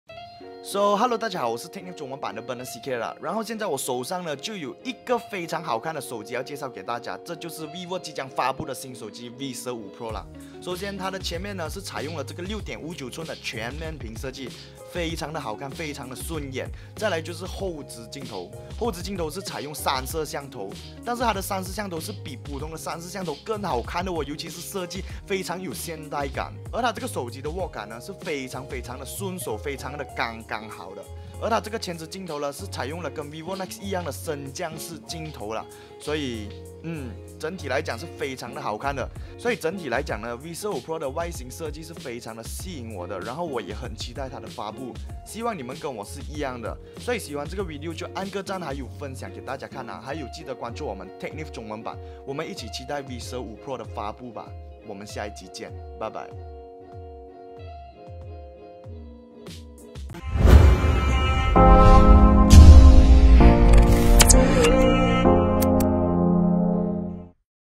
vivo V15 Pro—超美的后置3摄像头设计！
让大家先看看即将发布的vivo V15 Pro外形设计吧！
你是不是也被它的外形吸引了呢？尤其是这个配色！